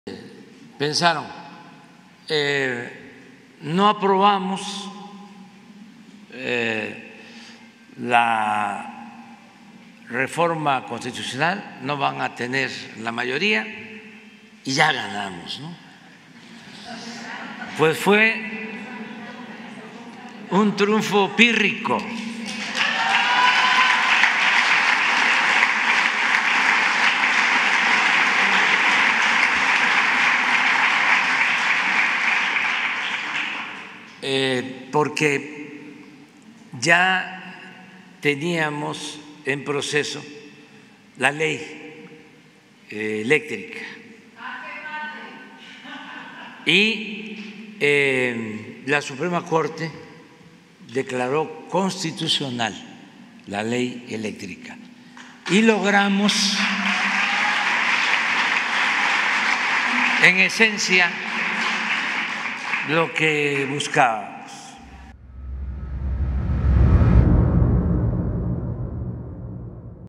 AMLO dice que la oposición celebró un triunfo pírrico
Video El presidente AMLO salió con todo y mencionó que la oposición pensó que si no aprobaban la reforma constitucional iban a ganar y pues no. ¡Zaz! 🤣🤣🤣👇👇👇